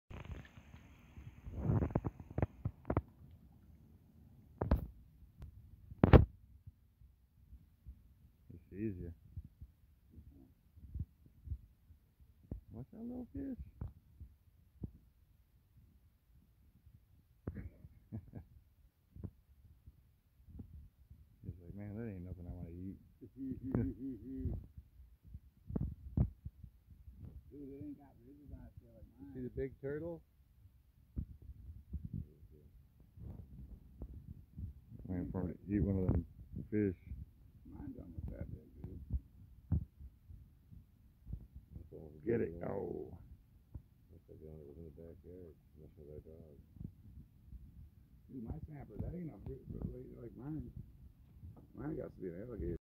Dinosaur Encounter
Met this old turtle at the local nature preserve today.